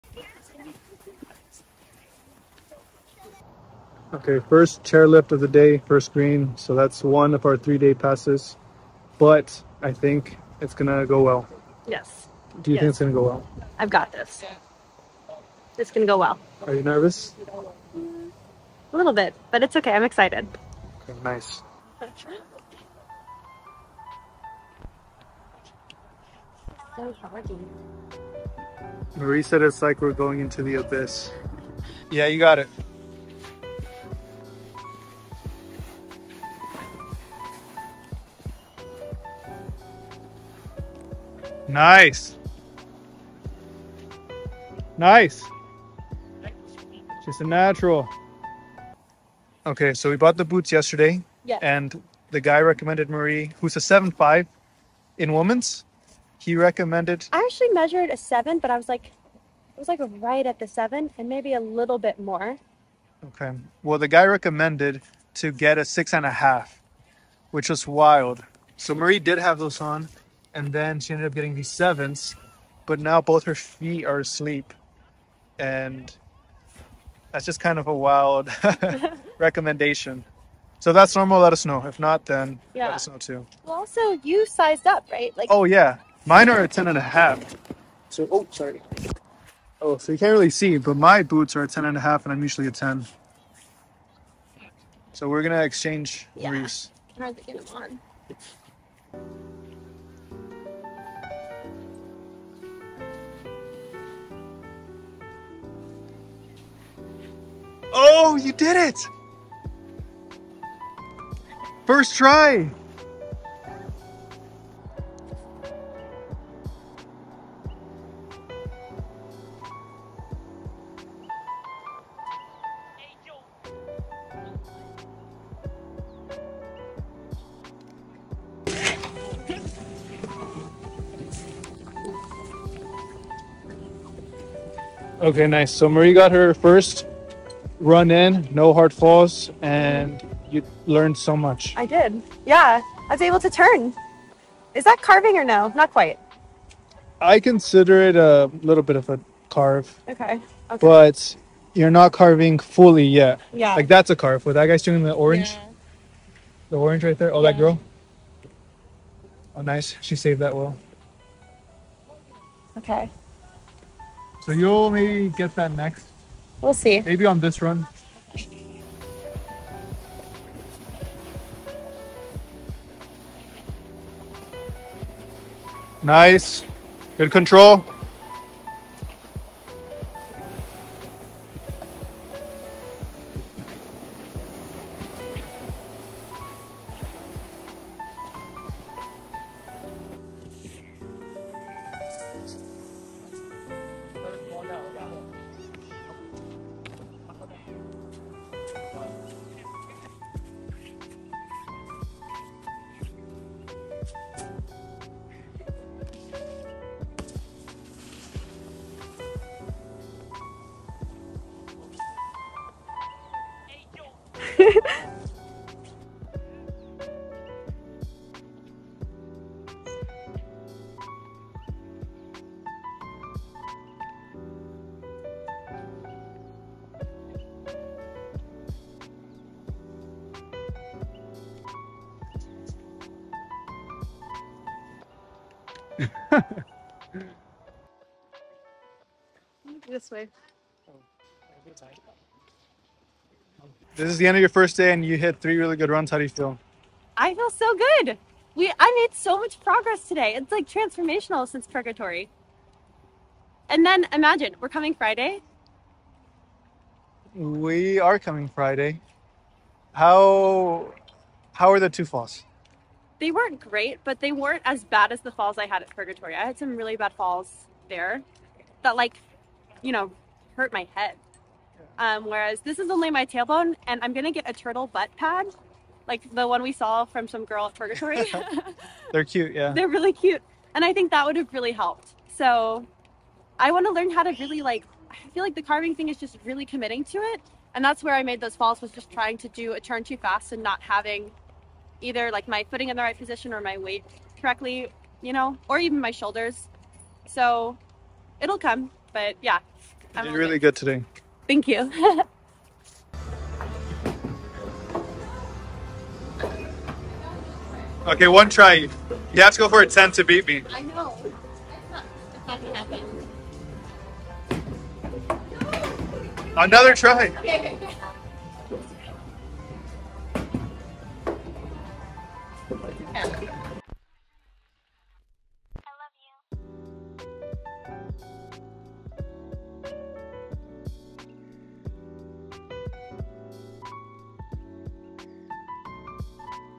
Snowboarding Snoqualmie In Seattle Vlog
Marie and I went snowboarding at Snoqualmie Ski Resort in Washington State. It is about an hour drive for us coming from Seattle but it is easily worth the drive. We picked up a new snowboard + boots for Marie so she broke them in and also learned some intermediate snowboarding skills.

00:00 - Intro
00:32 - First Run
00:48 - Bad Boot Advice
01:39 - Learning How To Carve
02:14 - Recap
03:10 - Grinding
04:03 - Ending
05:03 - Date Night